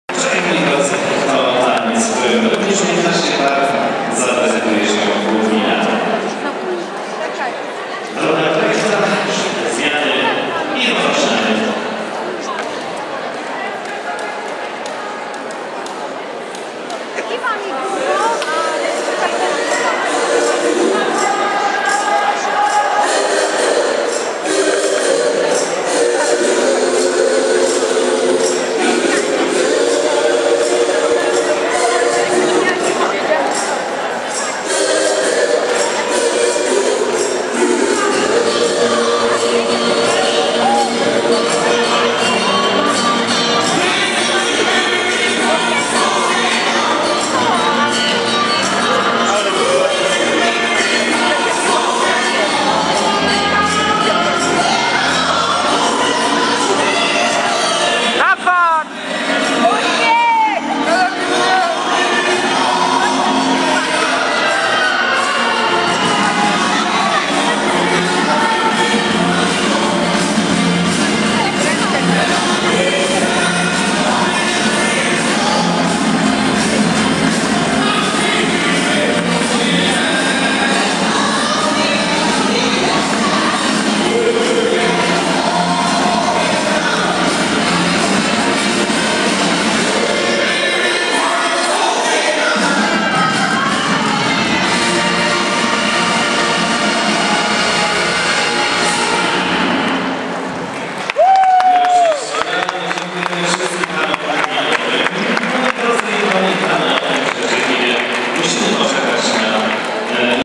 XXXIX Ogólnopolski Turniej Tańca Towarzyskiego o Puchar prof. Mariana Wieczystego
Jakub Datoń i Wiktoria Borkowska - Jive - 1/2 finału